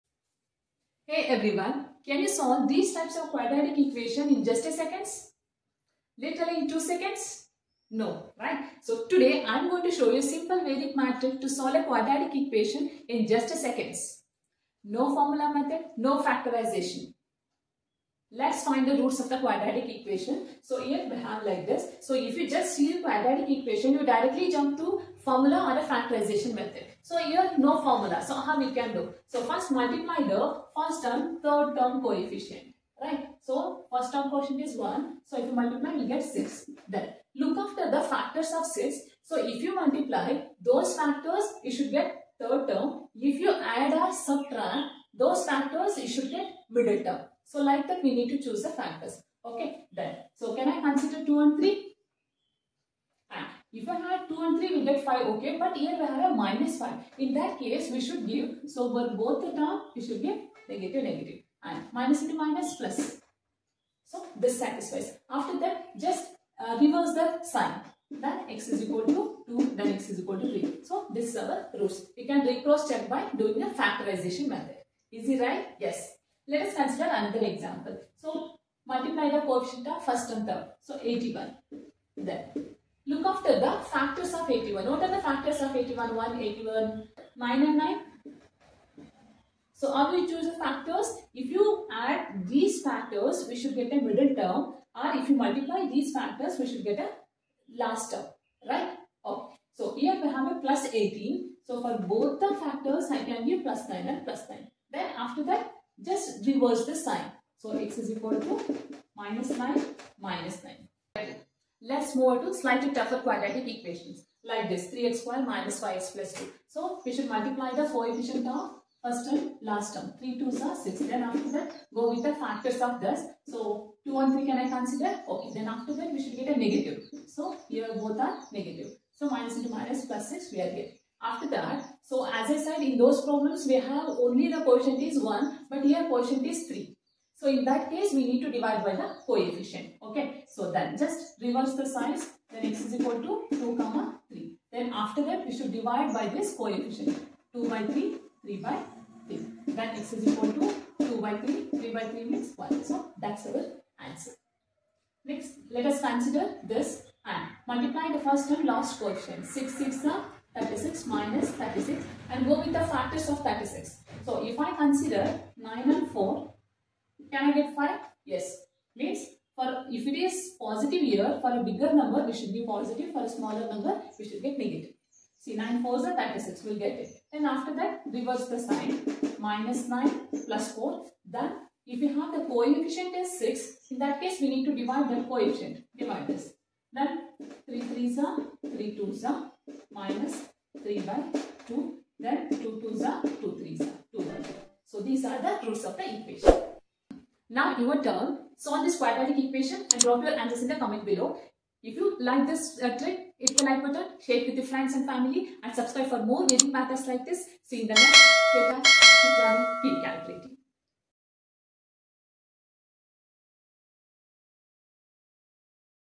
🔥 Solve ANY Quadratic in 5 Seconds! Vedic Maths Trick (No Formula Needed)
Learn how to solve quadratic equations in just 5 seconds using a powerful Vedic Math trick! No need for the quadratic formula, completing the square, or long calculations.

In this video, I’ll break down:
✅ The fastest method to solve quadratics
✅ Step-by-step examples (from easy to tricky!)
✅ How to recognize patterns instantly

Whether you're a student or a math lover, this method will save you time and boost your confidence!

📌 Watch till the end to master it completely and impress your friends!